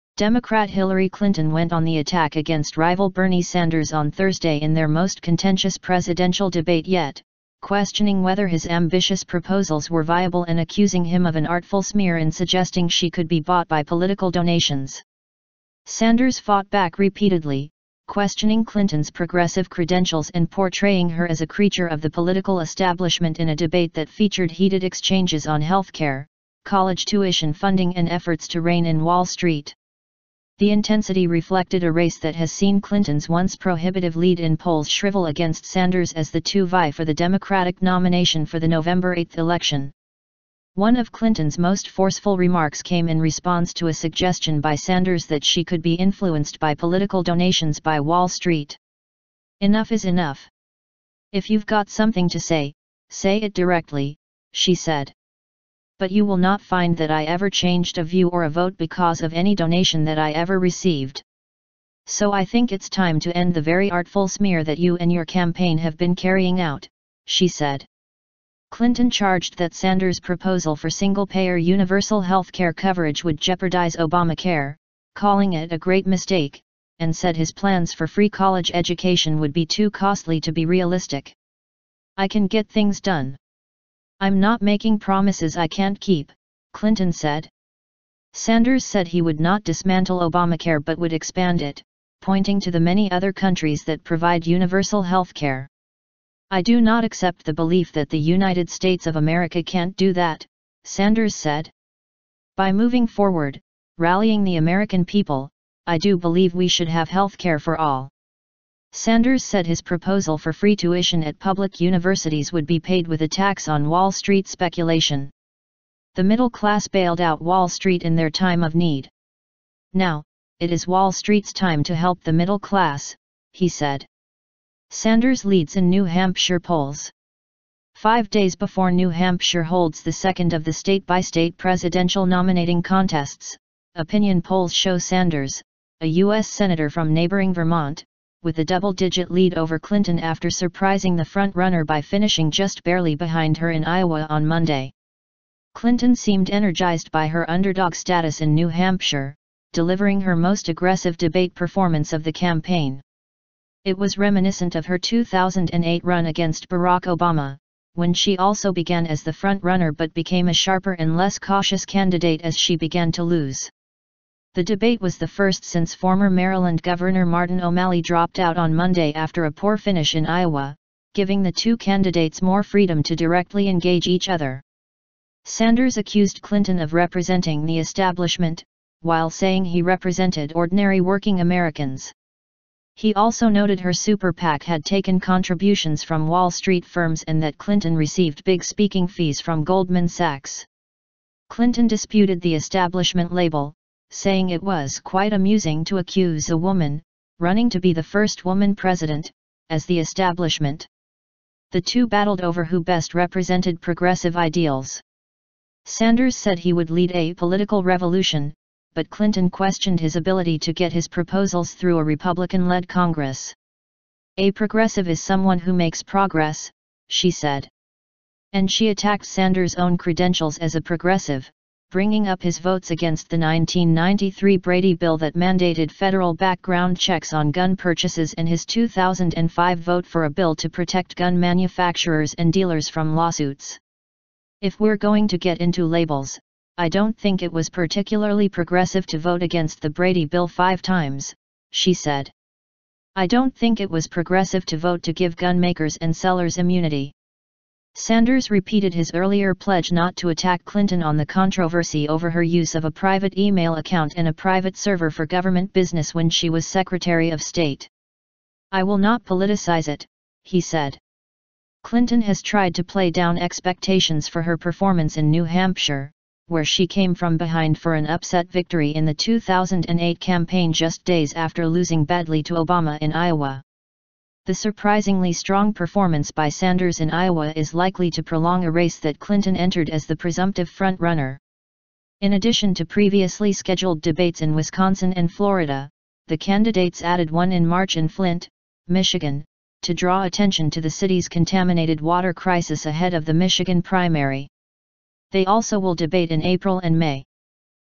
In contentious debate, Clinton and Sanders both claim 'progressive' mantle
Democrat Hillary Clinton went on the attack against rival Bernie Sanders on Thursday in their most contentious presidential debate yet, questioning whether his ambitious proposals were viable and accusing him of an "artful smear" in suggesting she could be bought by political donations.

Sanders fought back repeatedly, questioning Clinton's progressive credentials and portraying her as a creature of the political establishment in a debate that featured heated exchanges on healthcare, college tuition funding and efforts to rein in Wall Street.

The intensity reflected a race that has seen Clinton's once prohibitive lead in polls shrivel against Sanders as the two vie for the Democratic nomination for the Nov. 8 election.

One of Clinton's most forceful remarks came in response to a suggestion by Sanders that she could be influenced by political donations by Wall Street.

"Enough is enough. If you've got something to say, say it directly," she said. "But you will not find that I ever changed a view or a vote because of any donation that I ever received.

"So I think it's time to end the very artful smear that you and your campaign have been carrying out," she said.

Clinton charged that Sanders' proposal for single-payer universal healthcare coverage would jeopardize Obamacare, calling it "a great mistake," and said his plans for free college education would be too costly to be realistic.

"I can get things done. I'm not making promises I can't keep," Clinton said.

Sanders said he would not dismantle Obamacare but would expand it, pointing to the many other countries that provide universal healthcare.

"I do not accept the belief that the United States of America can’t do that," Sanders said. "By moving forward, rallying the American people, I do believe we should have healthcare for all."

Sanders said his proposal for free tuition at public universities would be paid with a tax on Wall Street speculation. "The middle class bailed out Wall Street in their time of need. Now, it is Wall Street's time to help the middle class," he said.

SANDERS LEADS IN NEW HAMPSHIRE POLLS

Five days before New Hampshire holds the second of the state-by-state presidential nominating contests, opinion polls show Sanders, a U.S. senator from neighboring Vermont, with a double-digit lead over Clinton after surprising the front-runner by finishing just barely behind her in Iowa on Monday.

Clinton seemed energized by her underdog status in New Hampshire, delivering her most aggressive debate performance of the campaign. It was reminiscent of her 2008 run against Barack Obama, when she also began as the front-runner but became a sharper and less cautious candidate as she began to lose.

The debate was the first since former Maryland Governor Martin O'Malley dropped out on Monday after a poor finish in Iowa, giving the two candidates more freedom to directly engage each other.

Sanders accused Clinton of representing "the establishment," while saying he represented "ordinary working Americans." He also noted her Super PAC had taken contributions from Wall Street firms and that Clinton received big speaking fees from Goldman Sachs.

Clinton disputed the establishment label, saying it was "quite amusing" to accuse "a woman, running to be the first woman president, as the establishment."

The two battled over who best represented progressive ideals. Sanders said he would lead a "political revolution," but Clinton questioned his ability to get his proposals through a Republican-led Congress.

"A progressive is someone who makes progress," she said.

And she attacked Sanders' own credentials as a progressive, bringing up his votes against the 1993 Brady bill that mandated federal background checks on gun purchases and his 2005 vote for a bill to protect gun manufacturers and dealers from lawsuits.

"If we're going to get into labels, I don't think it was particularly progressive to vote against the Brady bill five times," she said. "I don't think it was progressive to vote to give gun makers and sellers immunity."

Sanders repeated his earlier pledge not to attack Clinton on the controversy over her use of a private email account and a private server for government business when she was secretary of state.

"I will not politicize it," he said.

Clinton has tried to play down expectations for her performance in New Hampshire, where she came from behind for an upset victory in the 2008 campaign just days after losing badly to Obama in Iowa.

The surprisingly strong performance by Sanders in Iowa is likely to prolong a race that Clinton entered as the presumptive front-runner.

In addition to previously scheduled debates in Wisconsin and Florida, the candidates added one in March in Flint, Michigan, to draw attention to the city's contaminated water crisis ahead of the Michigan primary. They also will debate in April and May.